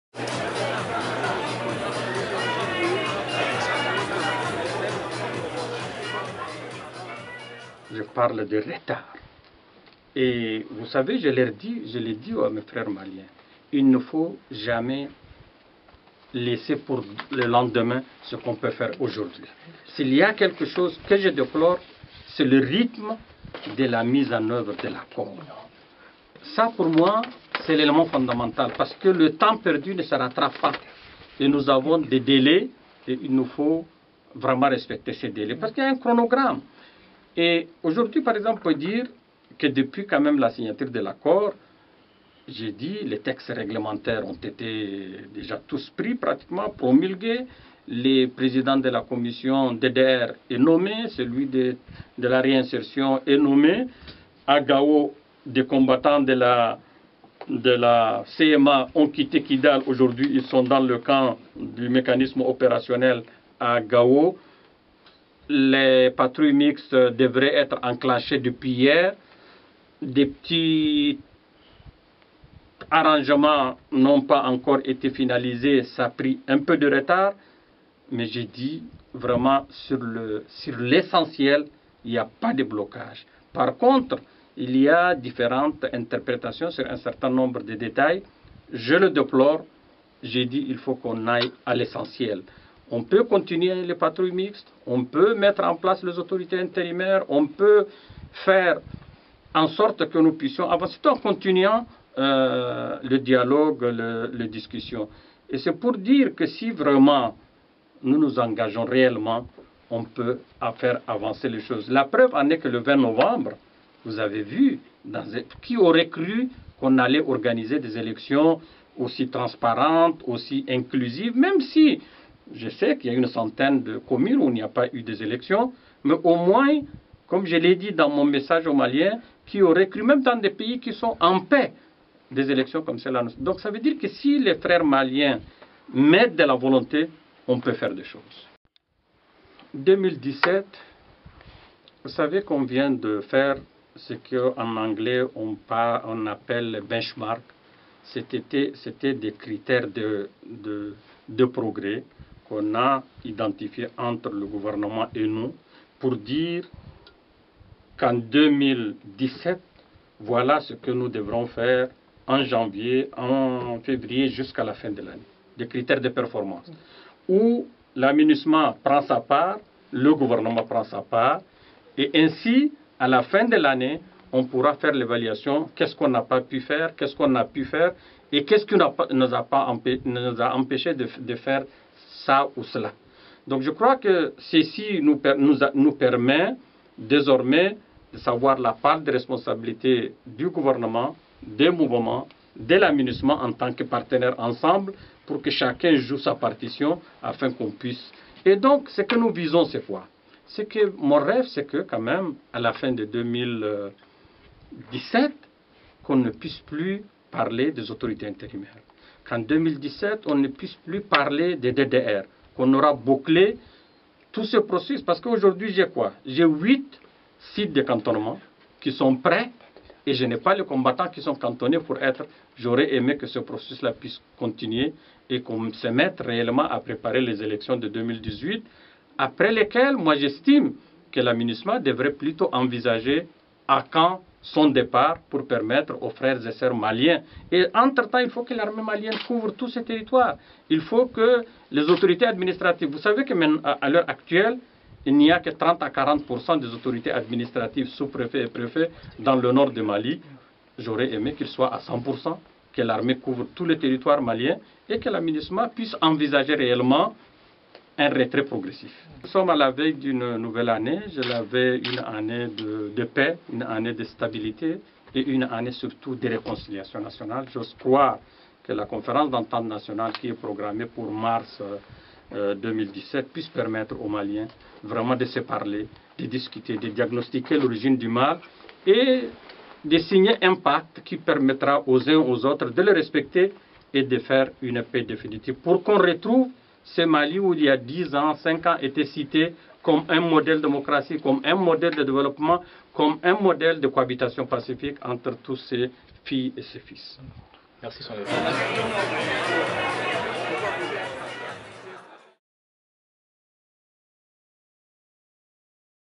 Mahamat Saleh Annadif, Bilan annuel
Mahamat Saleh Annadif, le représentant spécial du secrétaire général des Nations Unies, reconnait que des efforts restent à faire dans la mise en œuvre de l'accord pour la paix.